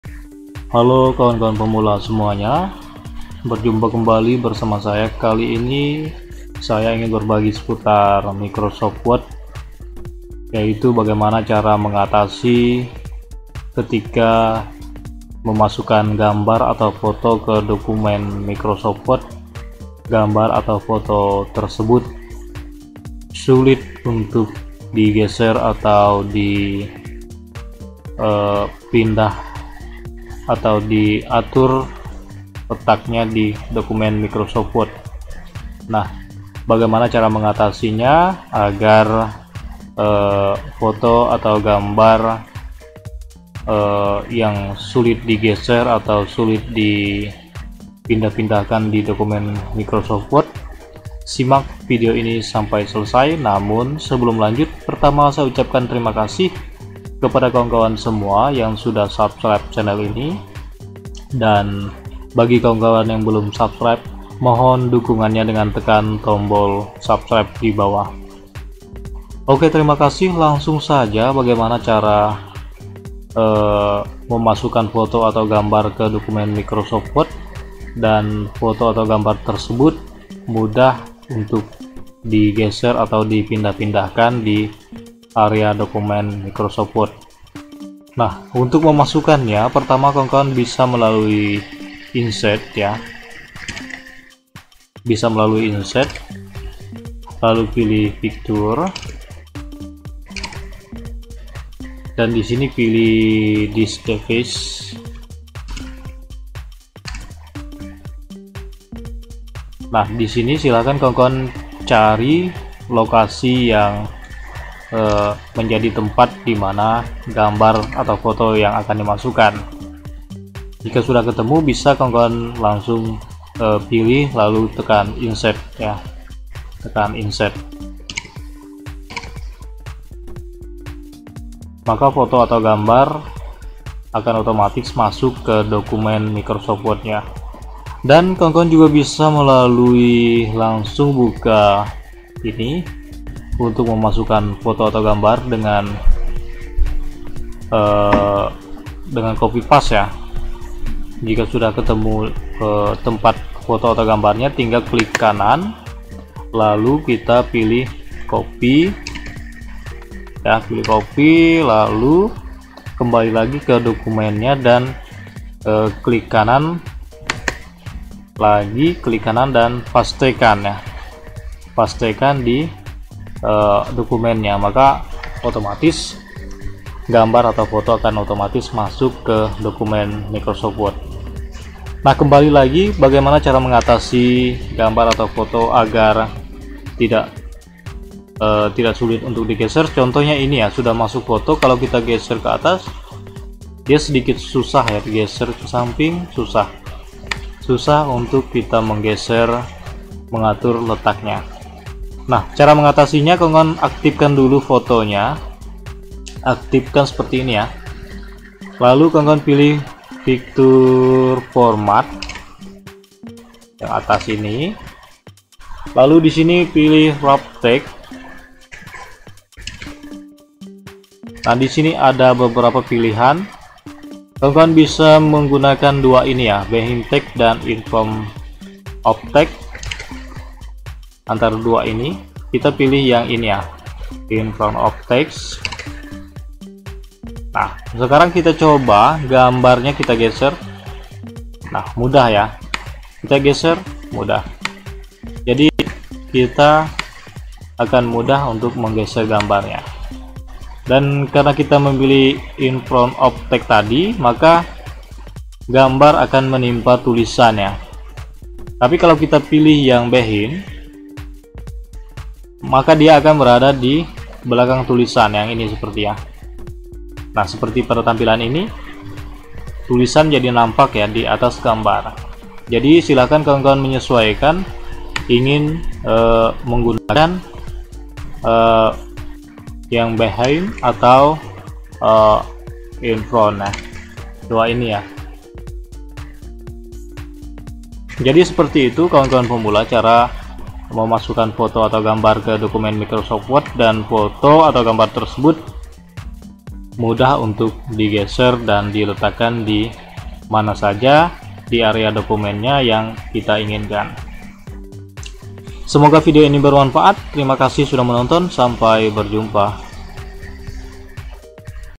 Cara mengatasi gambar tidak bisa digeser di word dengan gampang
Cara mengatasi gambar tidak bisa digeser di word, Gambar tidak bisa digeser di word,